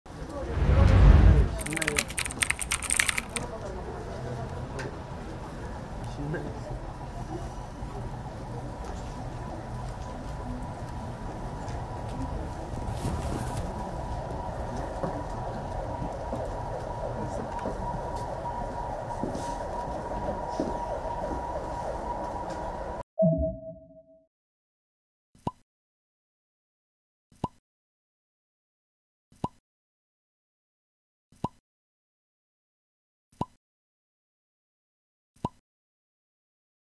[현장영상] '일용이' 박은수 황망한 듯 힘 없이 故김수미 찾았다 / 채널A
탤런트 김수미 씨가 오늘(25일) 오전 향년 75세로 별세했는데요. 전원일기에서 '일용' 역을 맡은 배우 박은수씨가 故김수미의 장례식장을 찾았습니다.